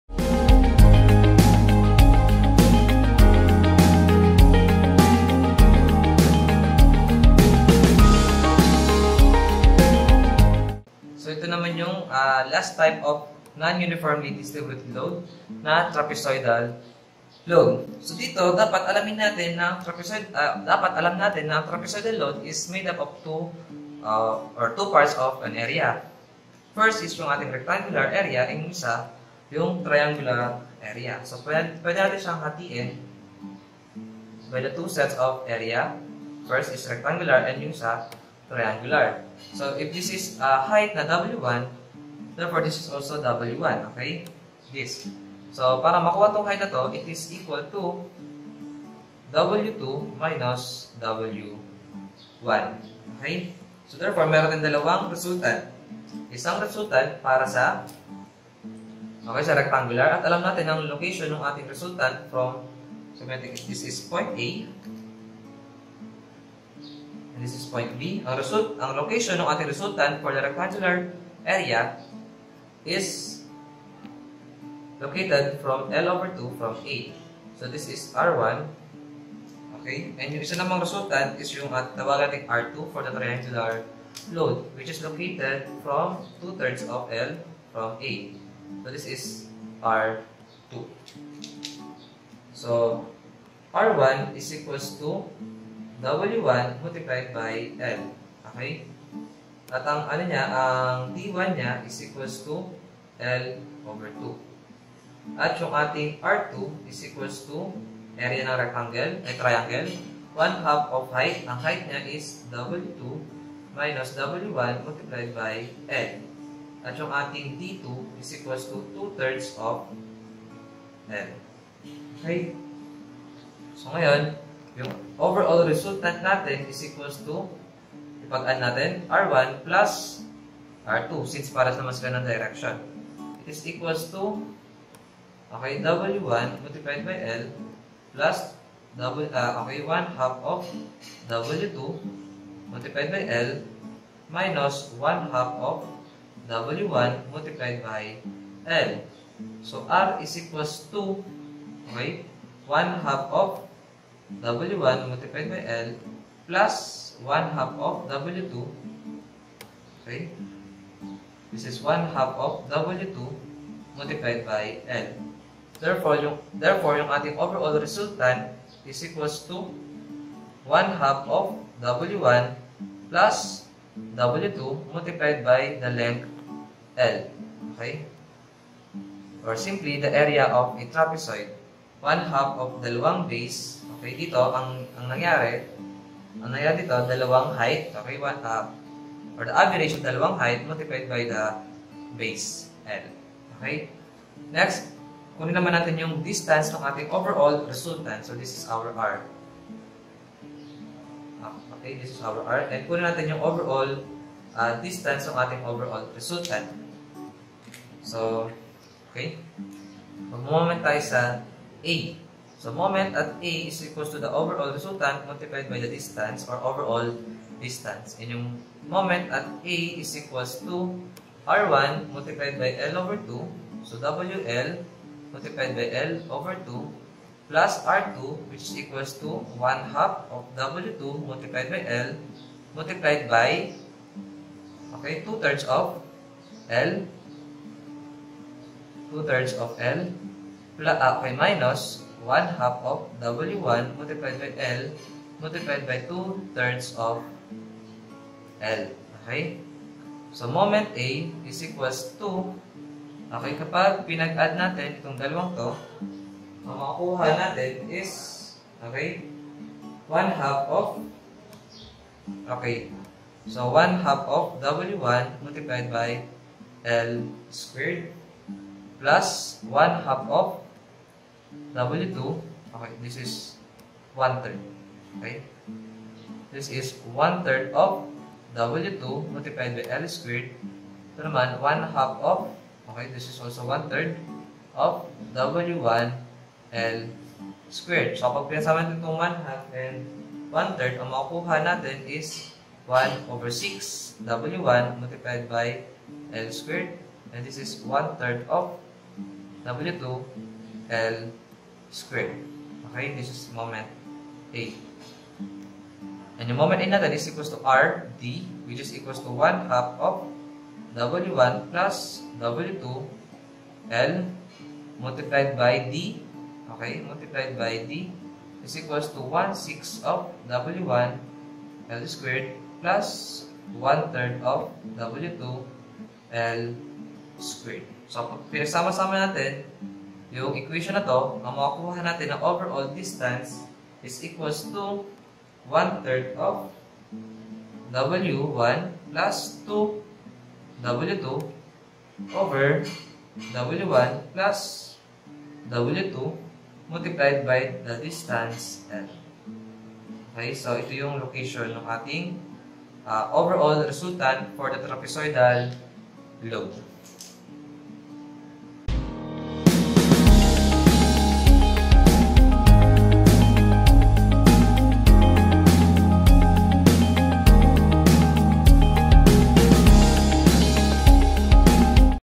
Statics of Rigid Bodies Chapter 2 Trapezoidal Distributed Load (PH)
Part I Statics of Rigid Bodies
Chapter II Resultant of Parallel Forces (Types of Load)
Trapezoidal Distributed Load

God bless! :)